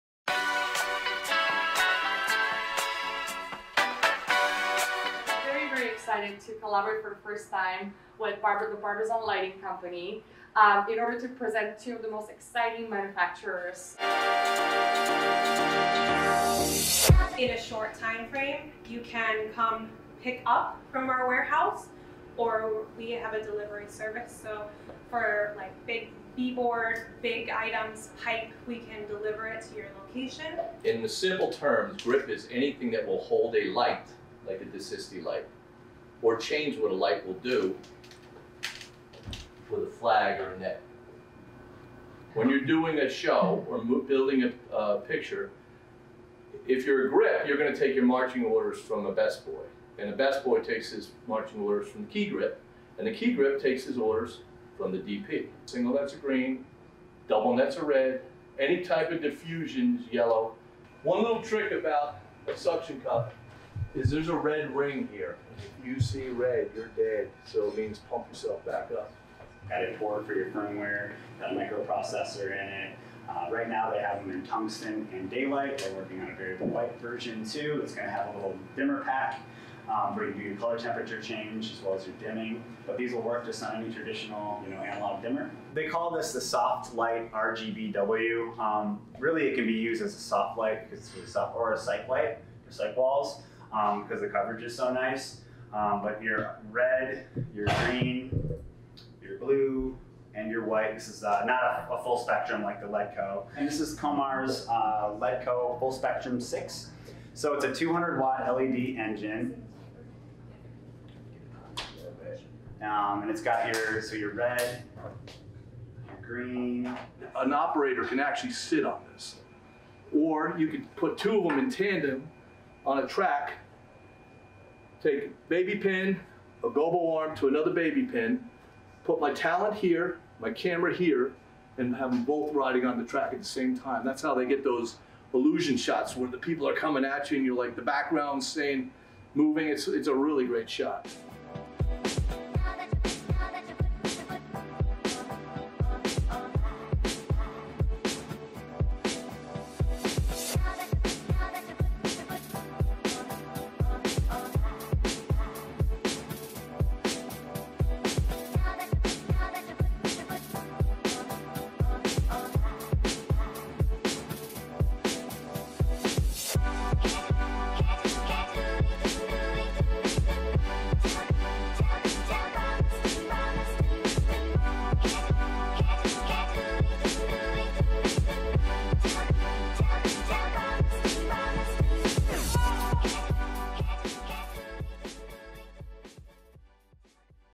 Cinematic Grip and Lighting LAB (with Barbizon, DeSisti, Coemar and Matthews Studio Equipment)
A sneak peek of what you can learn at our workshops. This time we bring you the Cinematic Grip and Lighting LAB where FilmGate Miami collaborate with this great lighting companies.